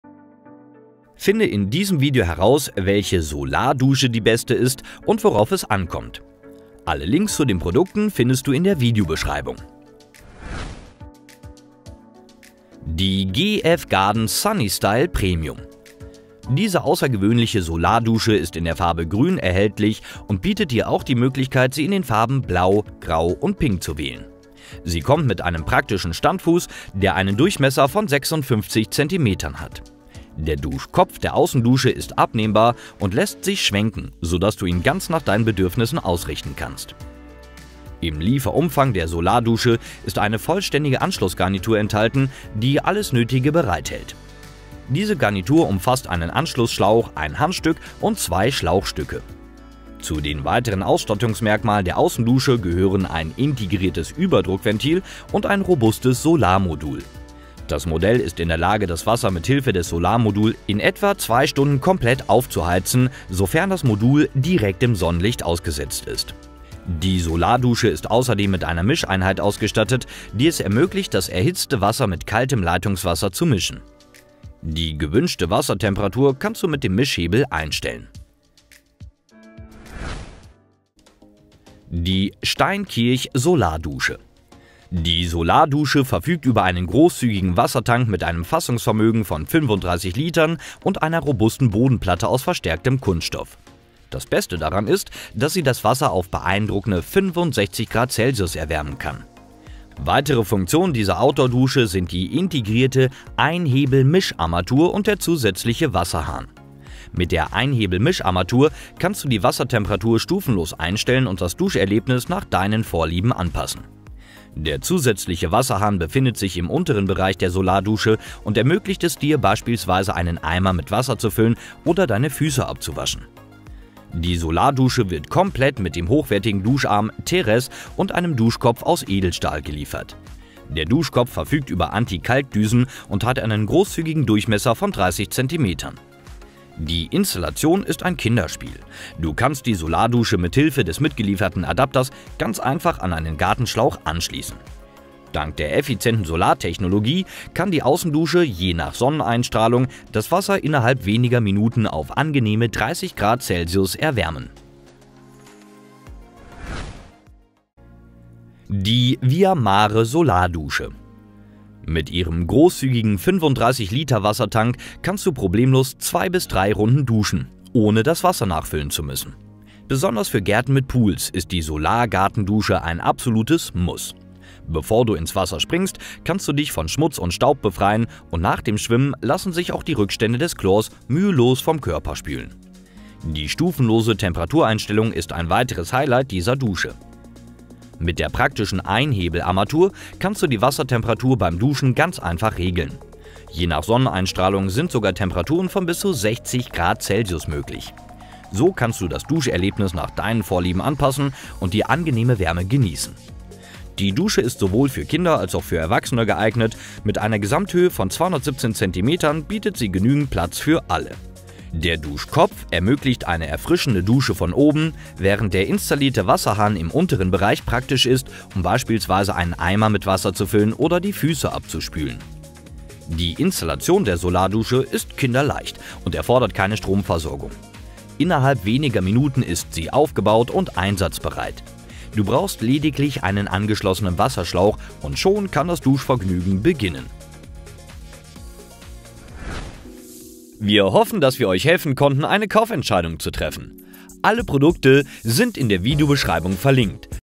Solardusche TEST (2025) - Die besten Solarduschen im VERGLEICH
Beste Solardusche im Test bzw. Vergleich 2025
★ Vergleichssieger ★
► STEINKIRCH Solardusche

★ Preis-Leistungs-Sieger ★
► GF GARDEN Sunny

★ Beste Bewertungen ★
► VIAMARE Solardusche

⏱🚨🚛 ⎮ 24H LIEFERUNG!

Welche Solardusche ist die Beste? In unserem Video zeigen wir euch die besten Solarduschen im Test bzw. Vergleich, welche es nach unserer Empfehlung auf Amazon zu kaufen gibt.

Wir haben unterschiedliche Hersteller verglichen, darunter die Solardusche von STEINKIRCH, eine Solardusche von GF GARDEN und die Solardusche von VIAMARE.

Liebe Grüße
Dein Vergleichssieger-Team

0:00 Intro
0:11 GF GARDEN Sunny Style Premium
1:20 STEINKIRCH-Solardusche
2:37 VIAMARE Solardusche